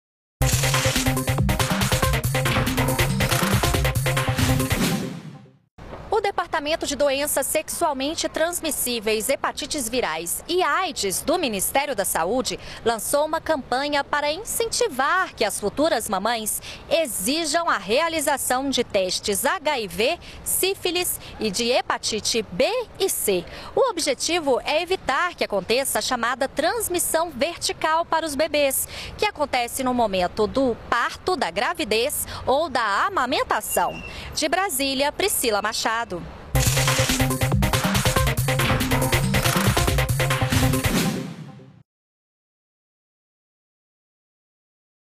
Ministério da Saúde lança campanha sobre a importância de testes de DSTs e aids no pré-natal
ÚLTIMAS - 07.05.15: O Departamento de DST, Aids e Hepatites Virais (DDAHV) do Ministério da Saúde lançou, nesta quarta-feira (6), uma campanha para incentivar grávidas a realizarem os testes dessas doenças durante o pré-natal. O objetivo é evitar a transmissão vertical (da mãe para o bebê) durante o parto, a gravidez ou a amamentação.